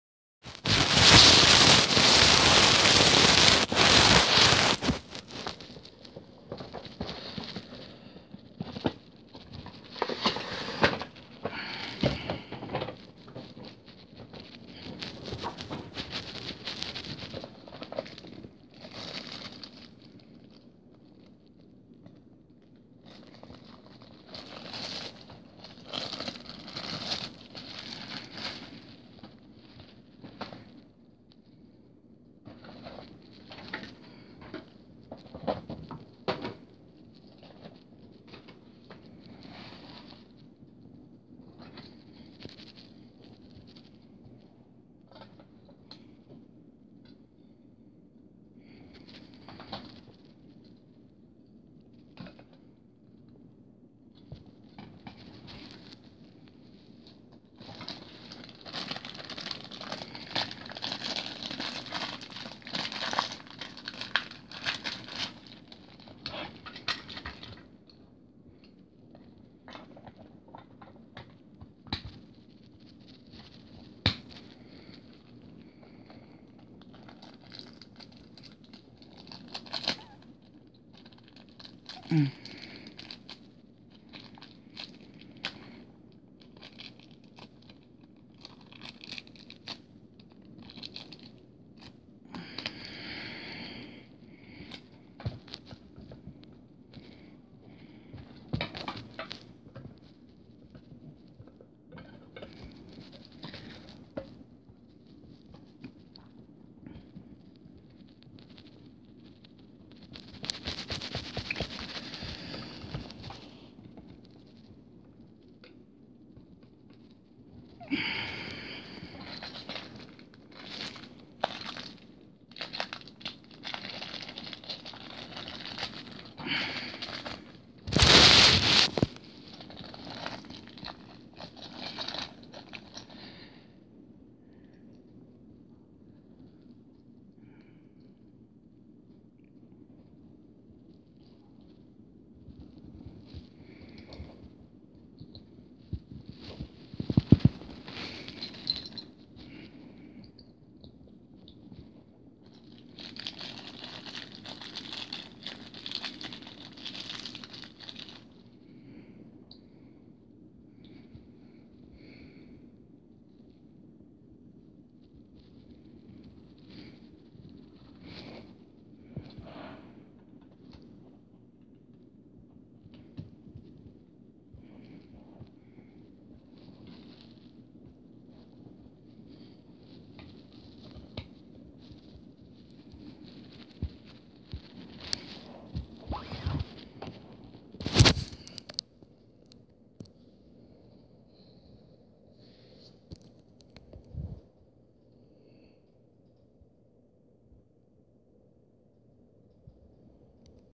Unboxing Skymarks Models LAN Boeing 767-300WL 1:150 W/Gear
I really love this airplane because it has the landing gear on the 767 aircraft and it's huge because it's a 1/150 scale and I really love this gift